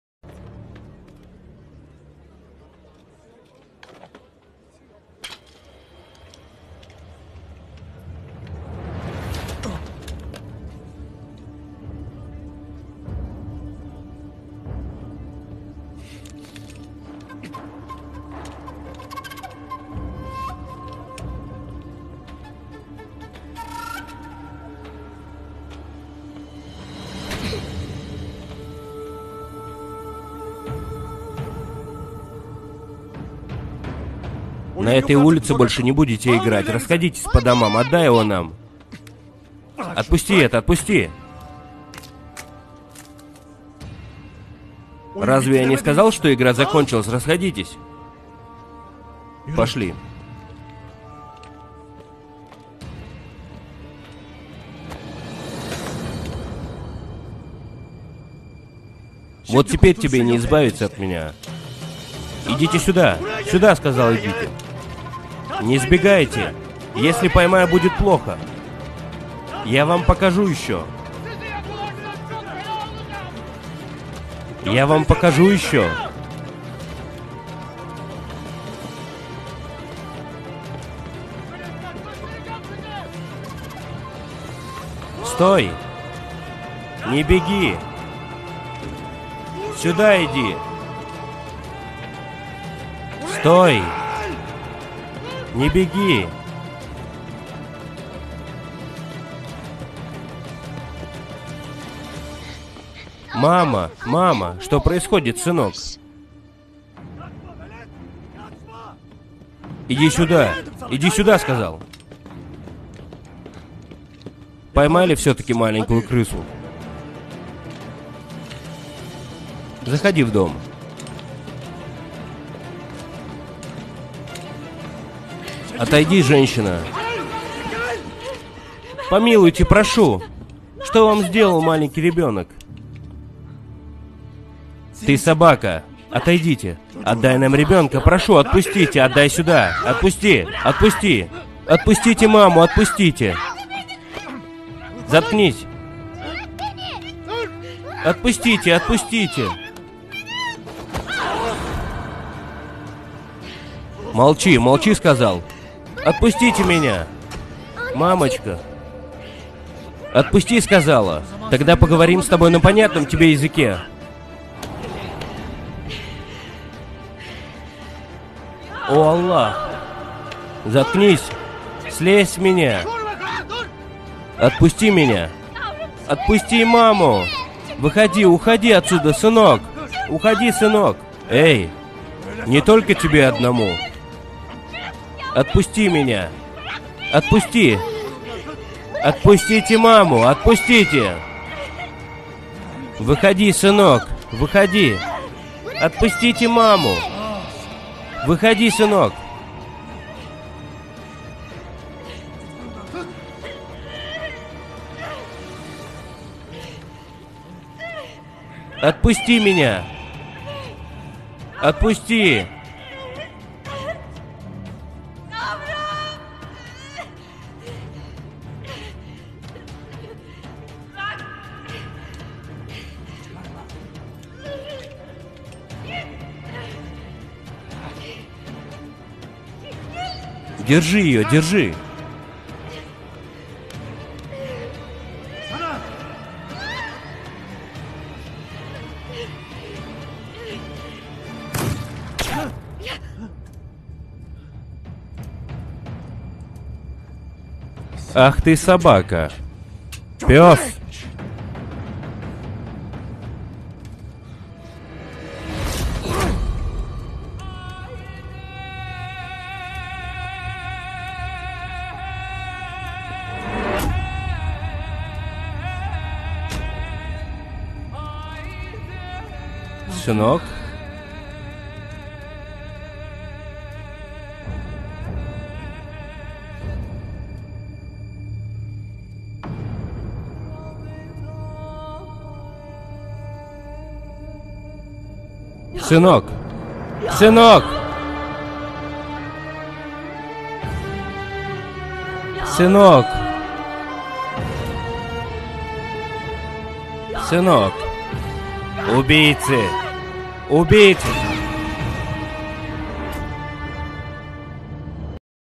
Judaxam tasirli video kórib fikrlaringizni kamentariyaga qoldiring
Judaxam taʼsirli va ayanchli ahvolga tushib qolgan bolakay

Salom Bizni kanalni kuzatib turgan yurtdoshla.

 Bu filim  qadimgi turk xoqonligi davrida boʻlib oʻtgan voqealar xaqida ajoyib qiziqarli serial

kino silkasini pastda oʻtib bemalol kirib koʻrishiz mumkin